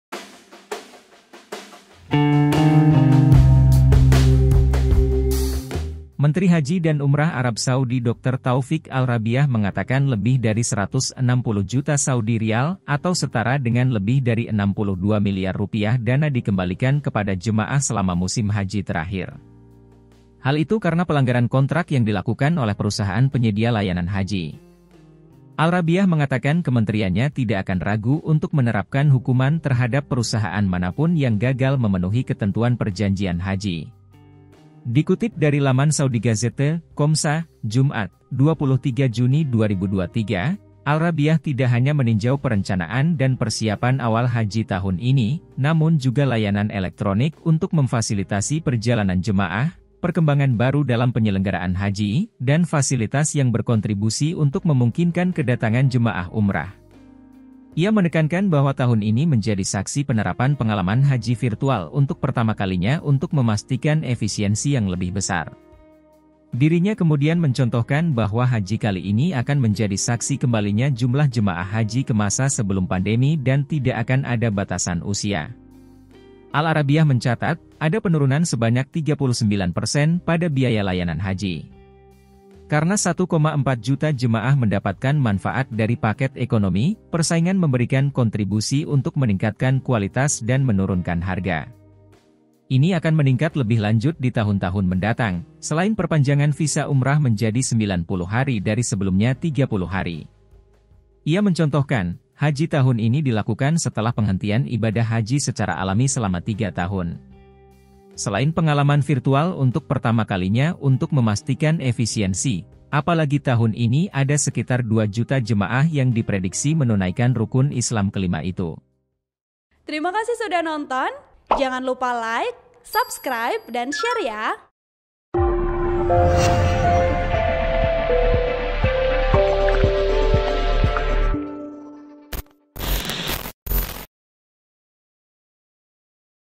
Terjadi Pelanggaran Kontrak, Penyedia Layanan Haji Kembalikan Rp 62 Miliar Dana kepada Jemaah Haji!
Laporan Wartawan Tribunnews, Fitri Wulandari
TRIBUN-VIDEO.COM, JEDDAH - Menteri Haji dan Umrah Arab Saudi Dr Tawfiq Al-Rabiah mengatakan lebih dari 160 juta Saudi Riyal atau setara dengan lebih dari Rp 62 miliar dana dikembalikan kepada jemaah selama musim haji terakhir.

Hal itu karena pelanggaran kontrak yang dilakukan oleh perusahaan penyedia layanan haji.

Al-Rabiah mengatakan kementeriannya tidak akan ragu untuk menerapkan hukuman terhadap perusahaan manapun yang gagal memenuhi ketentuan perjanjian haji.

"Sejumlah besar pengamat menindaklanjuti pelaksanaan paket yang telah disepakati, dan sanksi akan diterapkan kepada perusahaan manapun yang tidak mematuhi pelaksanaan perjanjian yang ditandatangani," tegas Al-Rabiah saat berbicara pada konferensi pers untuk memberi pengarahan tentang persiapan dan pengaturan haji pada Kamis (22/6/2023).

"Kami tidak hanya melihat penalti, tetapi kami juga memperhatikan insentif dan kami bekerja untuk memotivasi serta menghormati setiap perusahaan yang memberikan layanan yang melebihi ekspektasi, kami memiliki mekanisme untuk mengukur tingkat kepuasan," ujarnya.

Dikutip dari laman saudigazette.com.sa, Jumat (23/6/2023), Al-Rabiah tidak hanya meninjau perencanaan dan persiapan awal haji tahun ini, namun juga layanan elektronik untuk memfasilitasi perjalanan jemaah, perkembangan baru dalam penyelenggaraan haji, dan fasilitas yang berkontribusi untuk memungkinkan kedatangan jemaah umrah.

Ia menekankan bahwa tahun ini menjadi saksi penerapan pengalaman haji virtual untuk pertama kalinya untuk memastikan efisiensi yang lebih besar.

Dirinya kemudian mencontohkan bahwa haji kali ini akan menjadi saksi kembalinya jumlah jemaah haji ke masa sebelum pandemi dan tidak akan ada batasan usia.

Al-Arabiah mencatat, ada penurunan sebanyak 39 persen pada biaya layanan haji.

Karena 1,4 juta jemaah mendapatkan manfaat dari paket ekonomi, persaingan memberikan kontribusi untuk meningkatkan kualitas dan menurunkan harga.

Al-Rabiah mengindikasikan bahwa jemaah haji dari 58 negara di Eropa, Amerika dan Australia diperbolehkan untuk mendaftar secara online melalui platform terpadu dalam tujuh bahasa, selain kemungkinan untuk memilih paket yang sesuai dengan keunggulan harga yang lebih murah.

Ia mengacu pada penurunan biaya asuransi komprehensif dari 235 menjadi 88 Saudi Riyal, tanpa mengubah layanan dan cakupan yang ada.

Dirinya menegaskan bahwa perkembangan yang terjadi dalam memfasilitasi kedatangan jemaah di antaranya peningkatan jumlah jemaah haji hingga 30 persen dari jumlah tertinggi yang dicapai sebelumnya.

Ini akan meningkat lebih lanjut di tahun-tahun mendatang, selain perpanjangan visa Umrah menjadi 90 hari dari sebelumnya 30 hari.

Ia mencontohkan, haji tahun ini dilakukan setelah penghentian ibadah haji secara alami selama tiga tahun.

Selain pengalaman virtual untuk pertama kalinya untuk memastikan efisiensi, apalagi tahun ini ada sekitar dua juta jemaah yang diprediksi menunaikan rukun Islam ke-5 itu.

Al-Rabiah mengatakan Kerajaan Saudi menghabiskan ratusan miliar riyal untuk melayani para tamu Allah SWT.

"Kami telah menyiapkan rencana yang jelas dan terkoordinasi untuk mengatur proses pengiriman jemaah Haji, dengan perusahaan haji memastikan kemudahan pergerakan antar tempat suci," kata Al-Rabiah.

Artikel ini telah tayang di Tribunnews.com dengan judul Rp 62 Miliar Dana Dikembalikan kepada Jemaah akibat Pelanggaran Kontrak Penyedia Layanan Haji,
Penulis: Fitri Wulandari